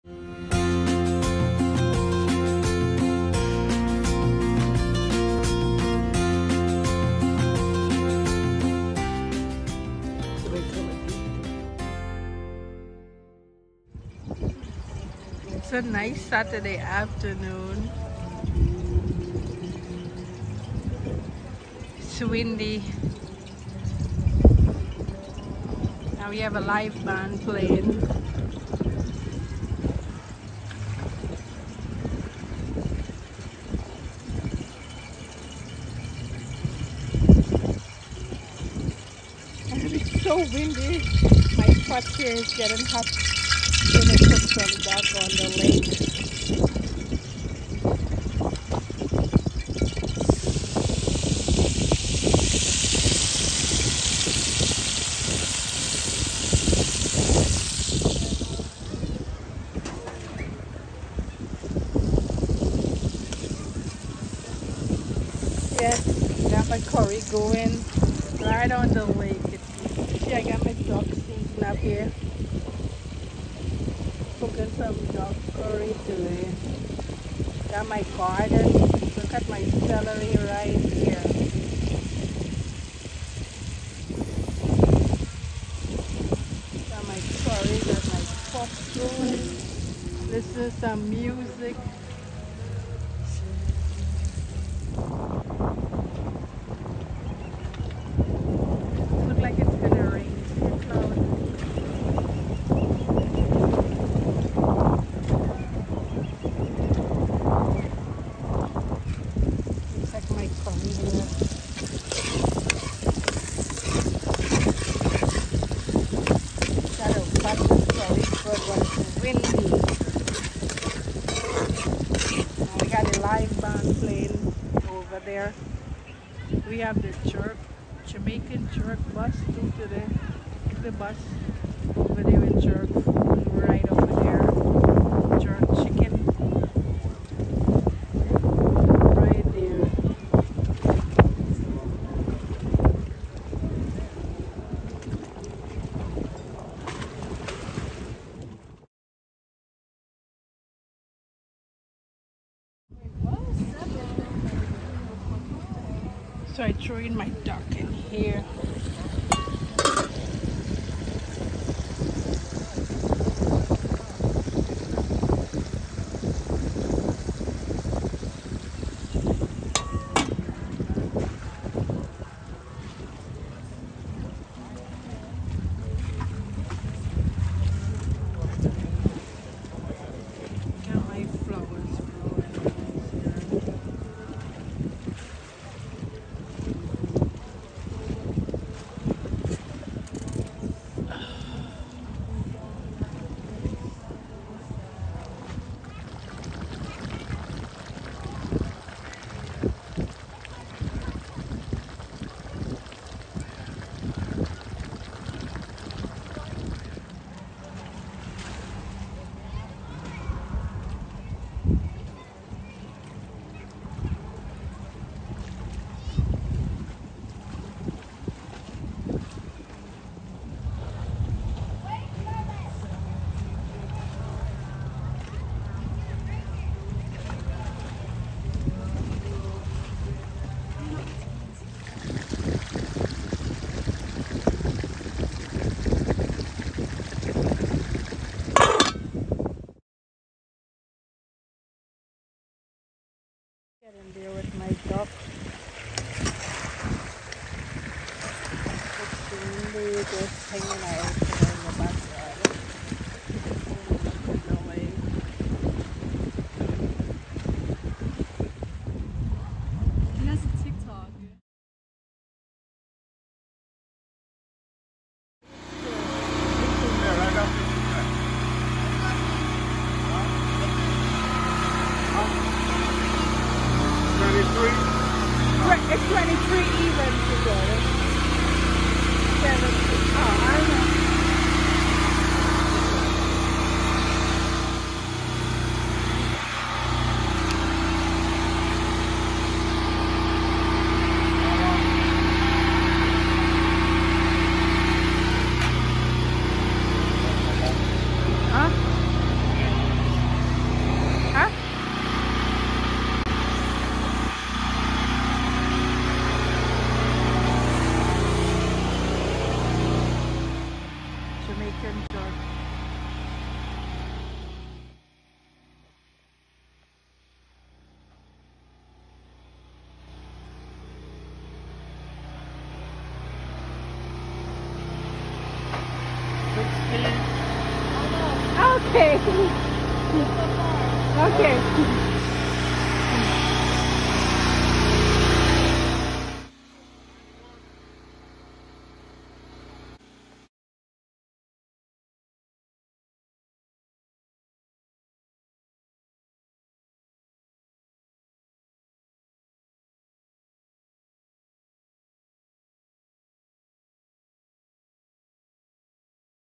Just chilling out on the lake#jerkchicken#food truck.
Relaxing on the lake cooking .